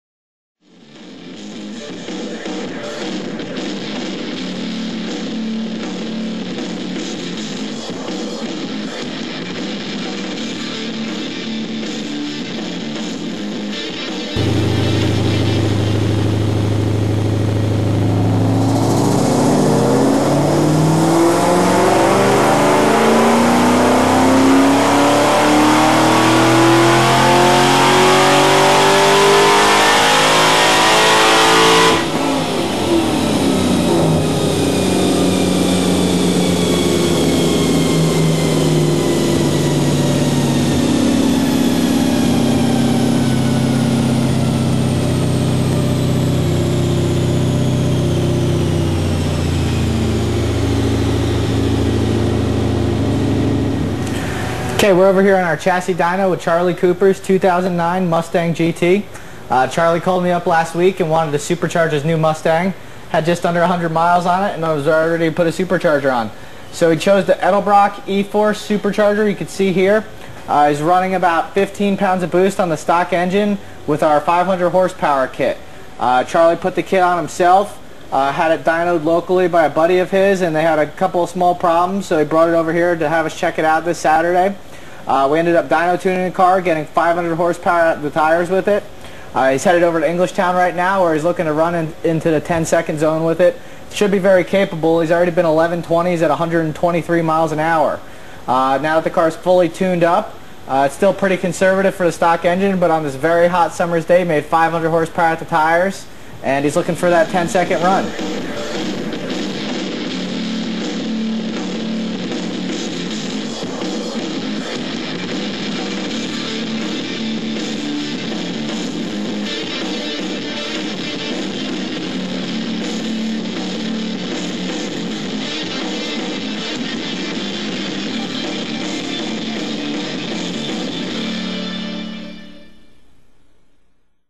another EDELBROCK 500HP E-Force Supercharger
on a new stock Mustang at JDM Engineering